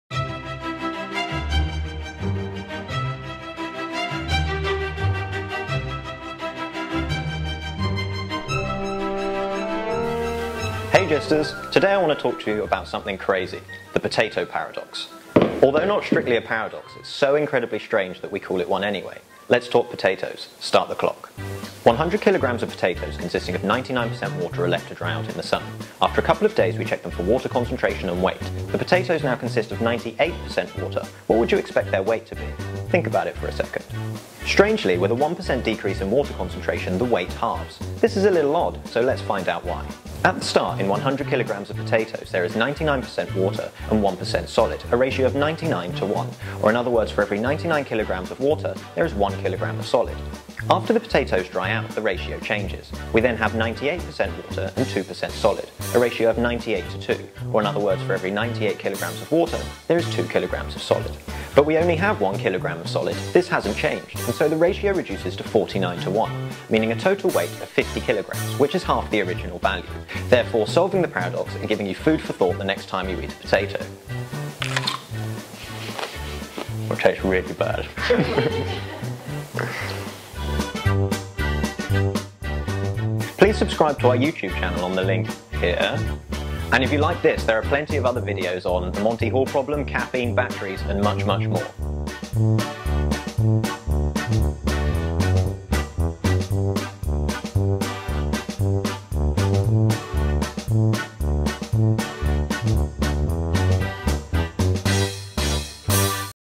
The Potato Paradox
Start with 100kg of potatoes consisting of 99% water, the potatoes dry out so that there is only 98% water, how much do they weigh? The answer is not as simple as you think... Find out the answer and why in the latest instalment of Just theGIST.